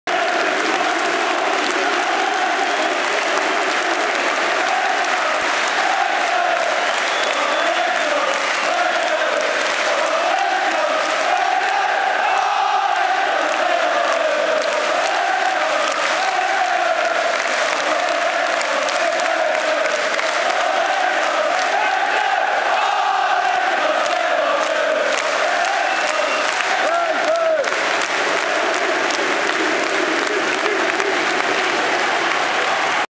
El Hadji Diouf Celebrates CIS Final 2011
El Hadji Diouf celebrates winning the CIS Cup.